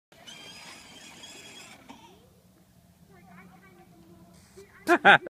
Chloe at the bottom of the hill at the park
Short video of Chloe getting to the bottom of the hill at the park, she then looks at daddy.